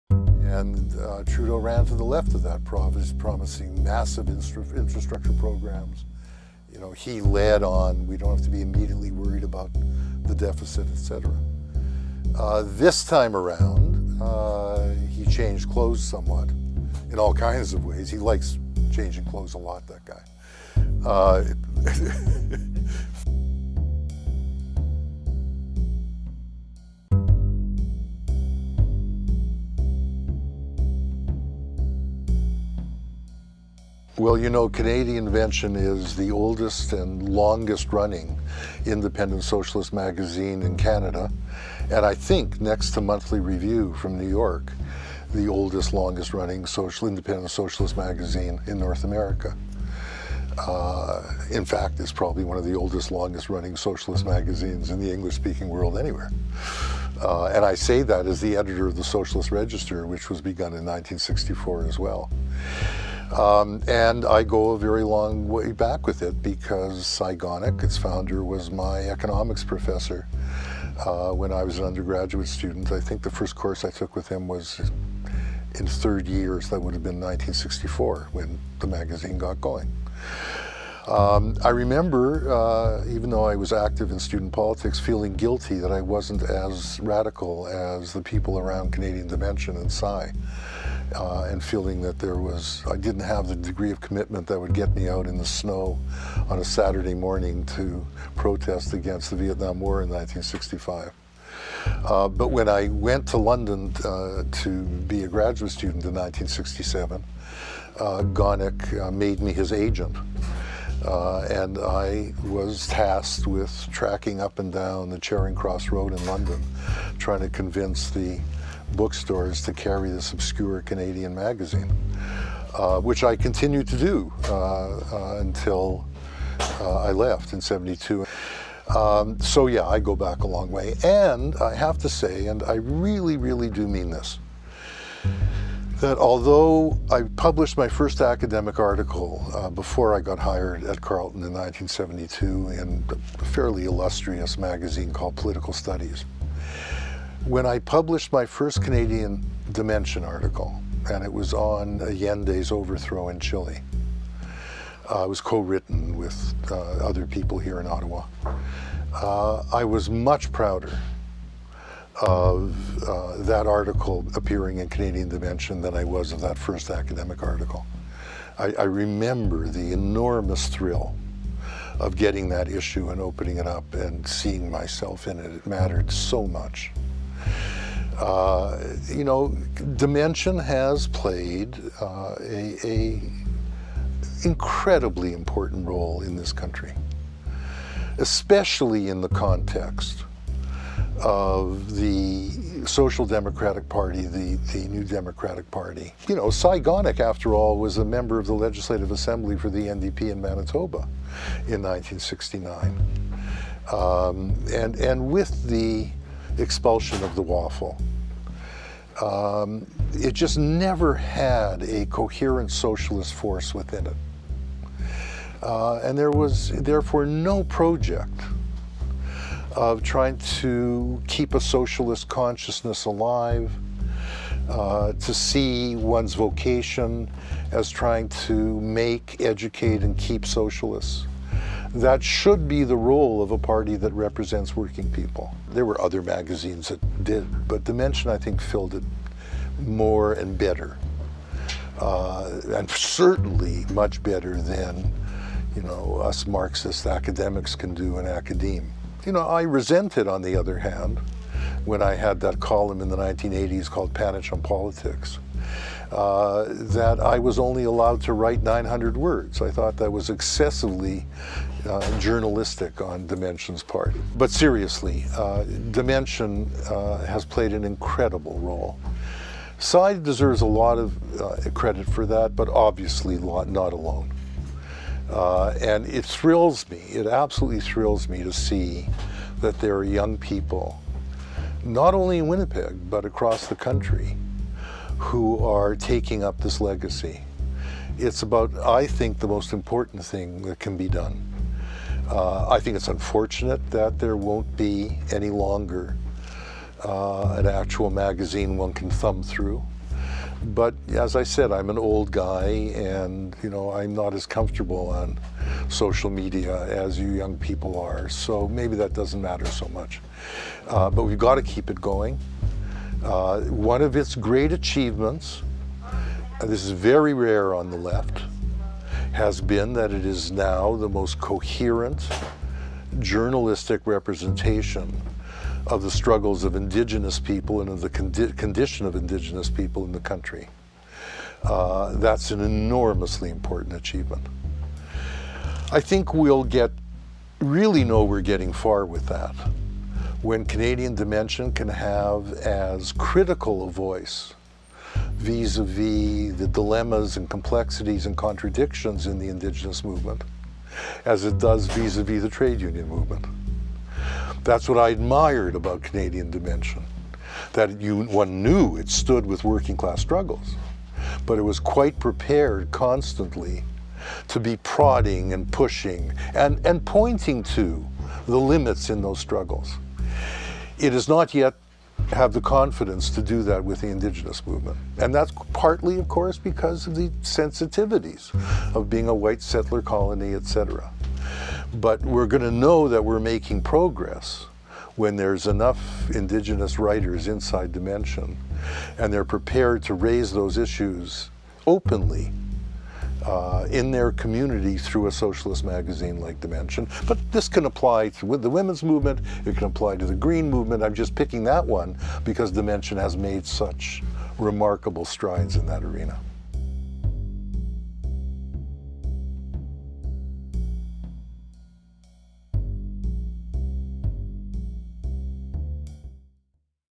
Leo Panitch - Canadian Dimension's Legacy on the Left
Leo Panitch reflects on Canadian Dimension's humble beginnings, its role in shaping left-wing thought in Canada, and shares his hopes for Dimension's digital future.

Special thanks to Black Squirrel Books Ottawa as always for allowing us to film in their beautiful shop blacksquirrelbooks.ca